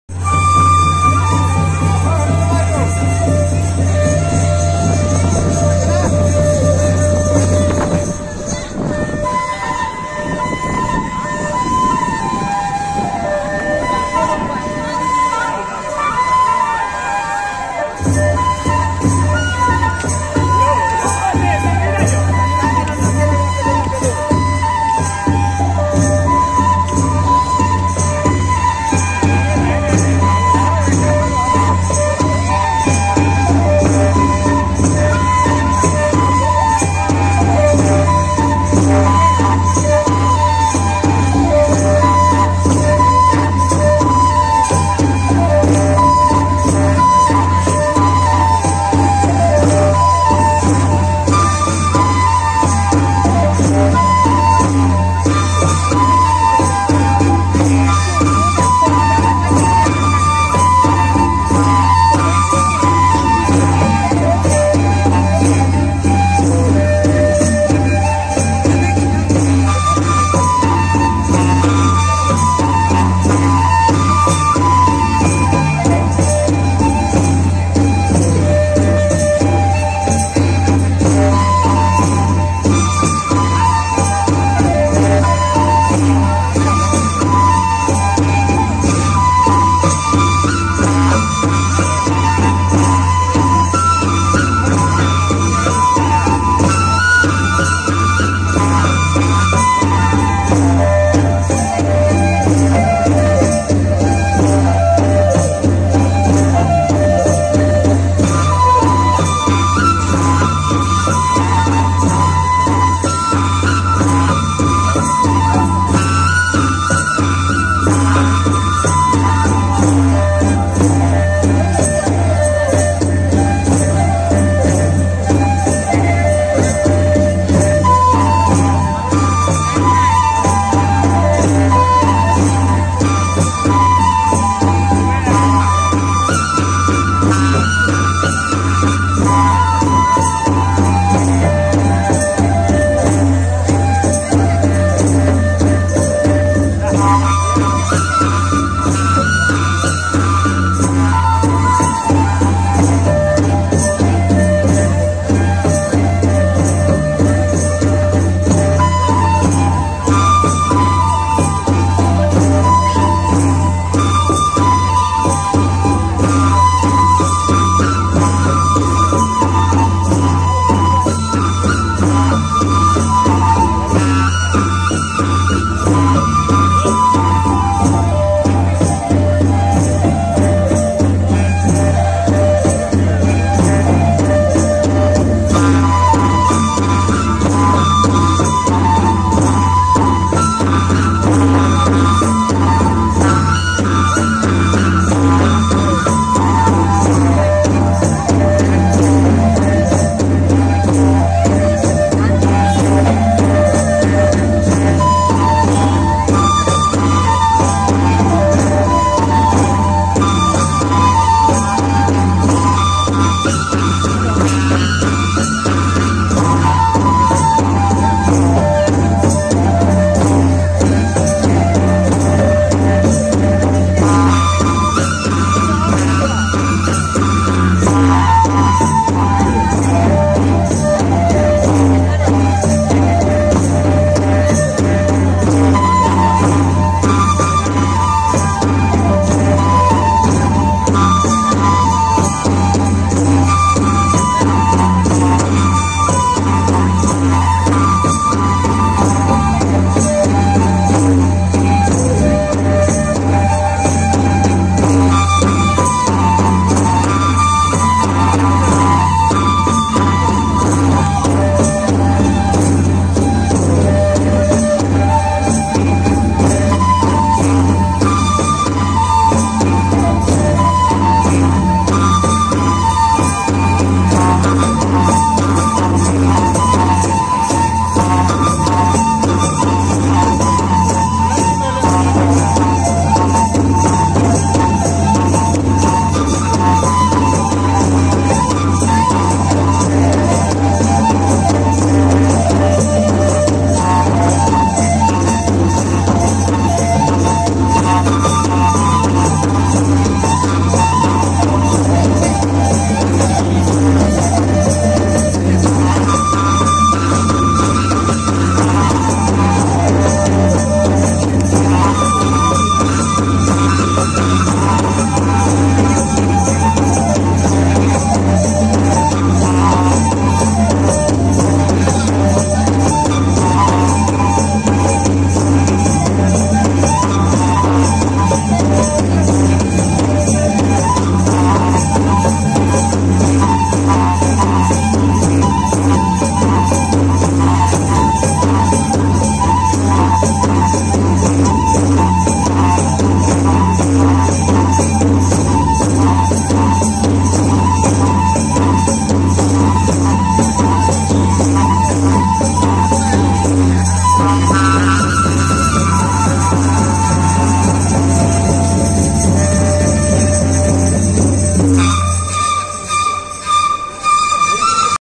Bagurumba Bihu Dance | Guwahati, Assam | Only Rare Footage.
Bagurumba Bihu Dance In Guwahati, Assam.

In this video:- Full Bagurumba Dance Video..

Type Of Video- Bihu Dance
Captured by - Me
Location - Gorchuk, Guwahati
Recorded By- Poco M2 Pro

Note- This Video Is Captured by me